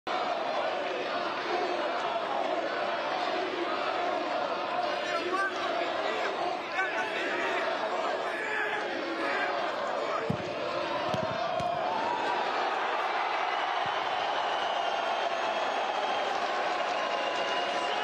ГОЛ⚽️ ЯРМОЛЕНКО! ДИНАМО - ВЕРЕС - 1:0
Андрій Ярмоленко відкриває рахунок в матчі